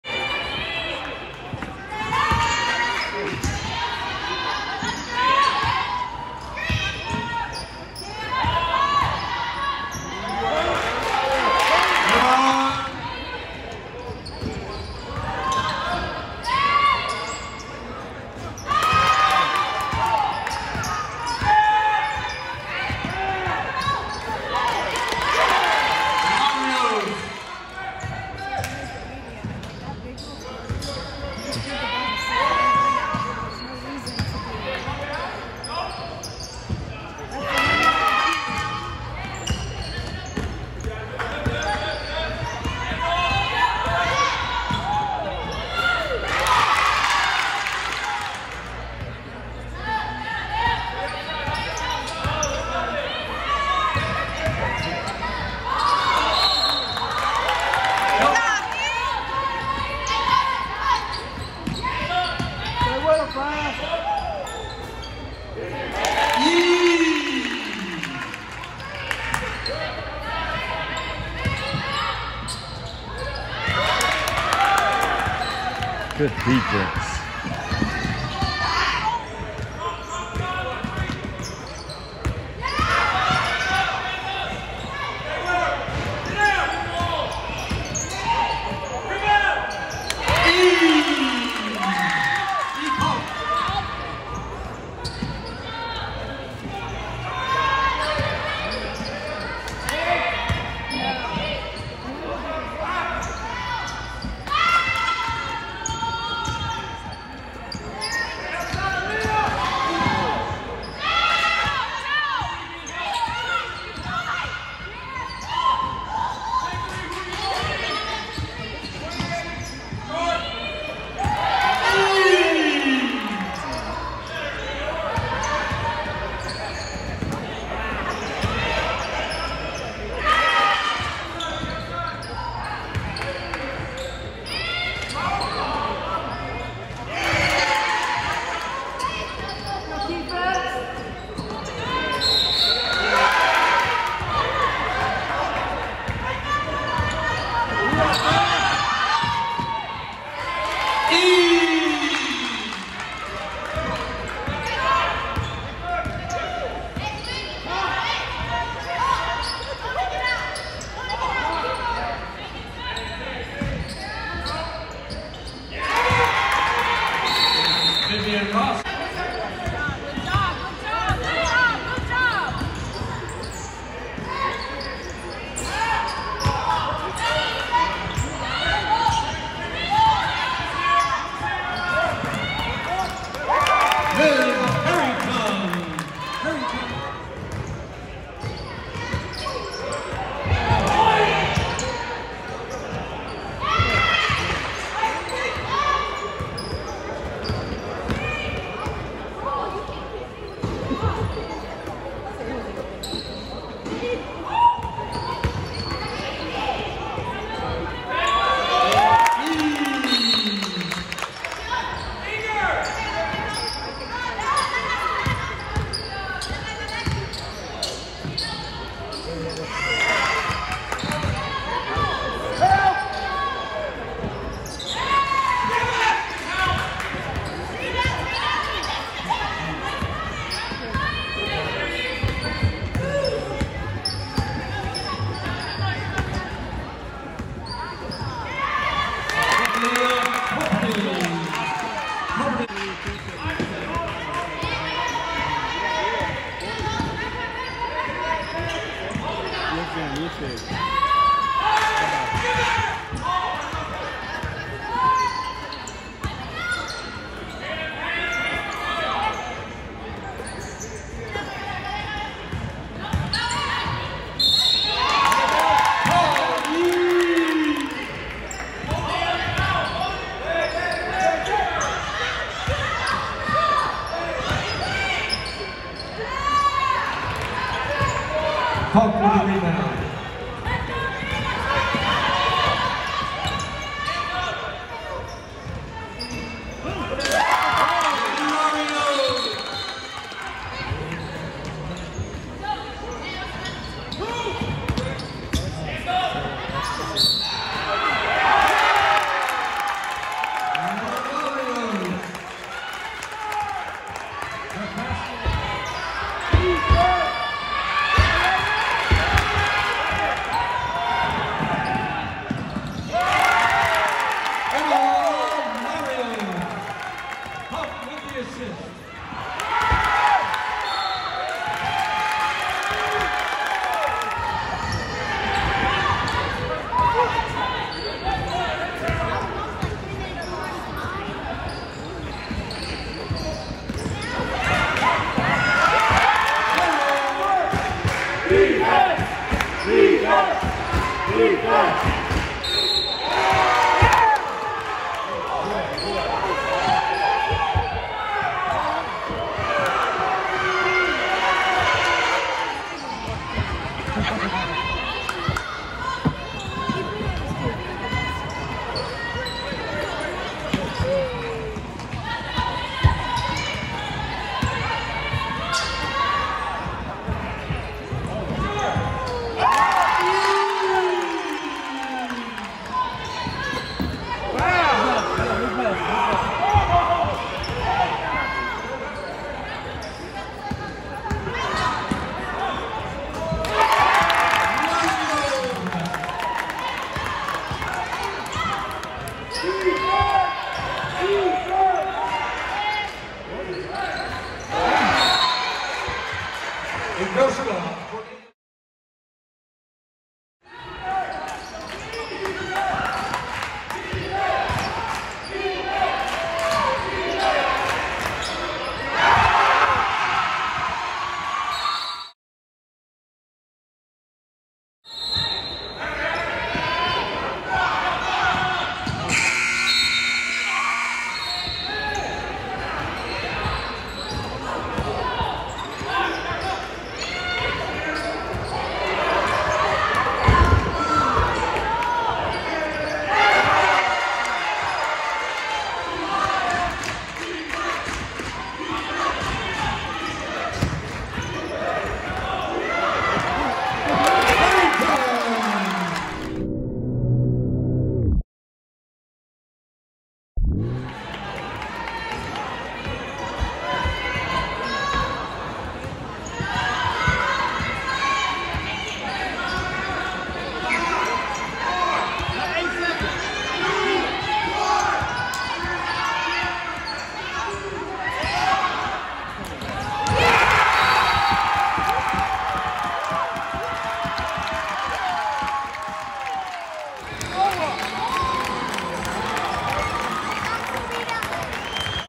BUZZER BEATER - ACA Girls basketball vs Rabun Gap High School
Asheville Christian Academy Girls Basketball